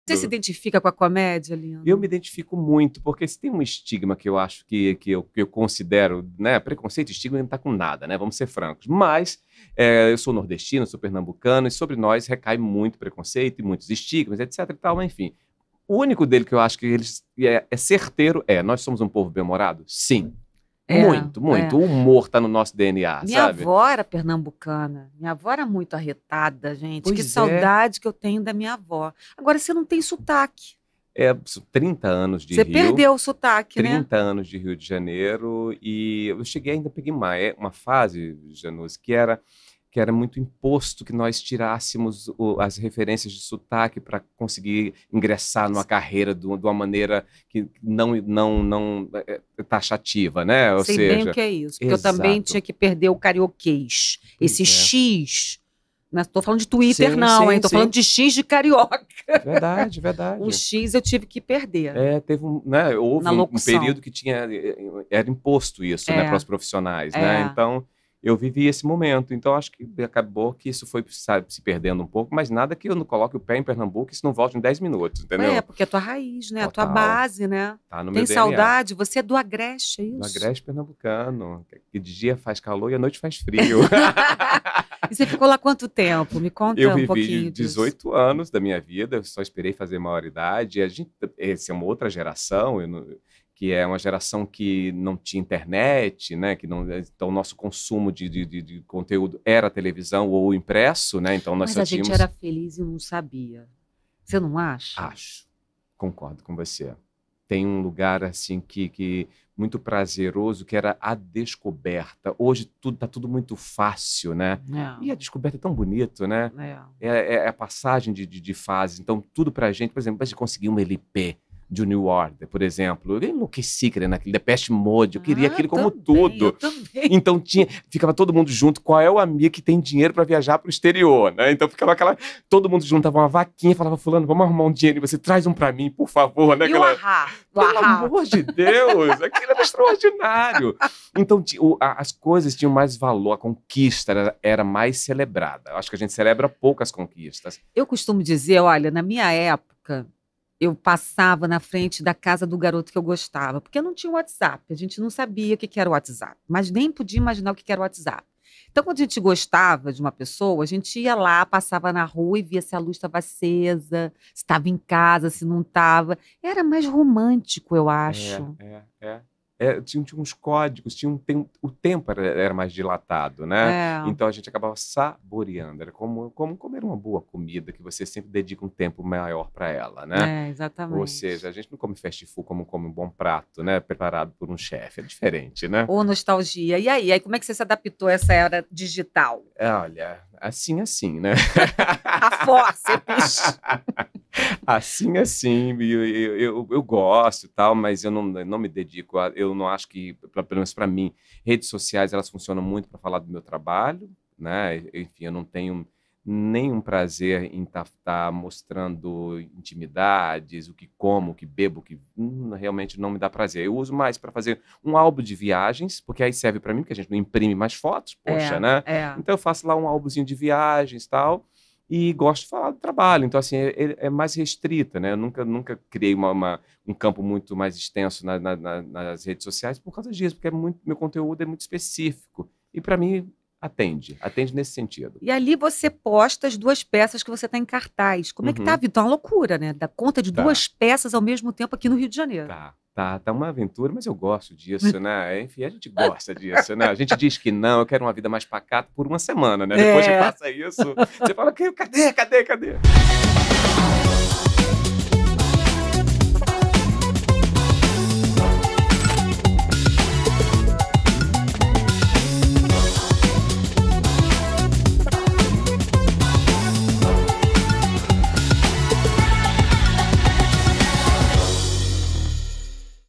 ALEXANDRE LINO FALA SOBRE A ERA DIGITAL E SE JÁ SE IDENTIFCAVA COM A COMÉDIA
No JannuzziEAbuzze34 conversei o ator e diretor, Alexandre Lino (@alexandrelinooficial)!

Alexandre Lino é um talentoso ator, produtor e diretor natural de Gravatá, Pernambuco. Bacharel em Cinema pela UNESA, com mestrado em Artes Cênicas pela UNIRIO, ele se destacou na cena artística carioca desde os anos 2000, quando iniciou sua carreira no Teatro Glória sob a gestão de Antônio Abujamra. Ao longo dos anos, Lino trabalhou em produções de sucesso na televisão, incluindo a Rede Record e a Rede Globo, participando de novelas aclamadas como "Amor Sem Igual" e "Um Lugar ao Sol".

No teatro, Lino consolidou sua carreira com atuações marcantes em peças como "Domésticas", que lhe rendeu uma indicação ao Prêmio Ítalo Rossi. Ele foi protagonista do documentário cênico "O Pastor", aclamado pela crítica, e idealizou o projeto "Nordestinos", que venceu o pitching do Tempo Festival em 2015. Seu solo "Lady Christiny" e a peça "O Porteiro" também conquistaram sucesso, com apresentações em mais de 40 cidades e reconhecimento em prêmios importantes.

Além de sua carreira como ator, Lino se destacou como diretor, trazendo à cena obras inovadoras como "Volúpia da Cegueira" e o musical "Chica da Silva". Com diversos prêmios em festivais de cinema, ele também dirigiu curtas e documentários, incluindo "Saudades Eternas". Atualmente, Lino continua em turnê pelo Brasil e planeja lançar novos projetos no cinema e teatro, enquanto ministra aulas e workshops sobre empreendedorismo artístico.